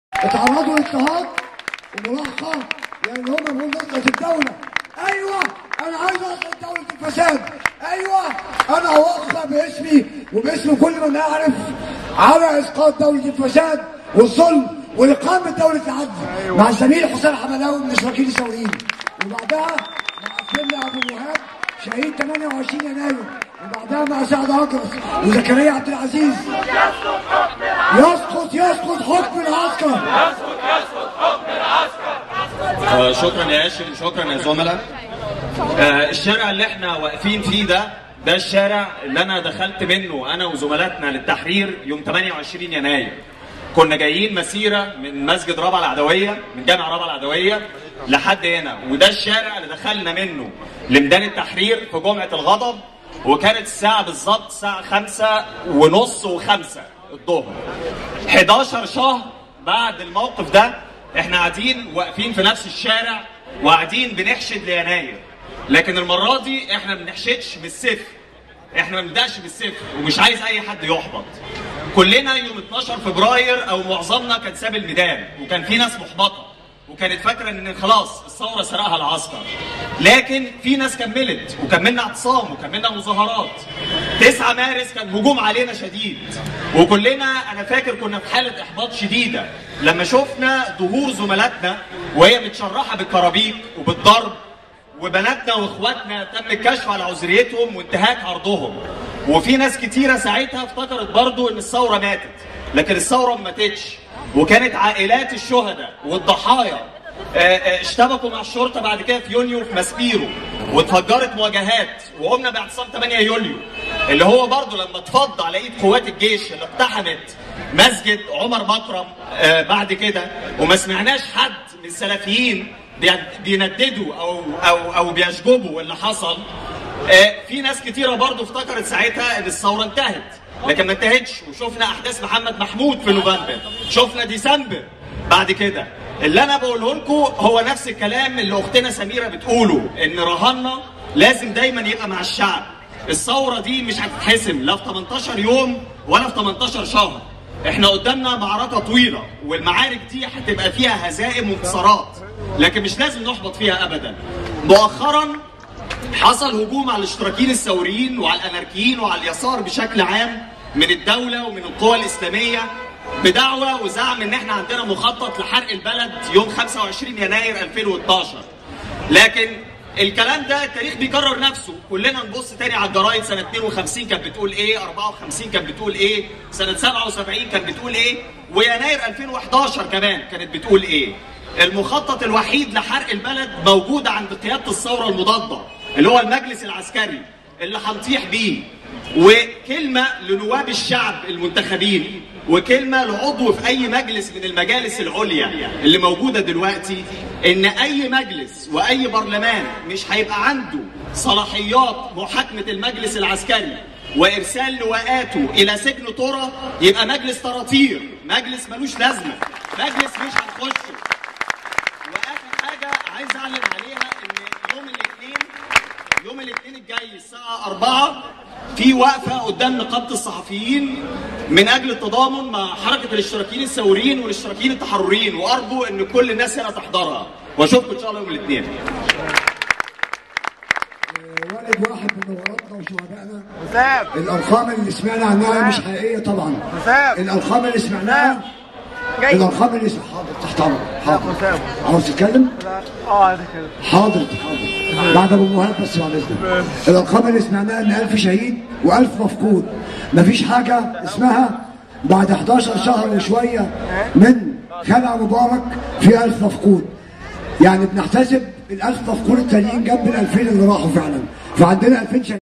حسام الحملاوي - حركة الاشتراكيين الثوريين
My speech at the solidarity conference in Qasr el-Nil Street, downtown Cairo.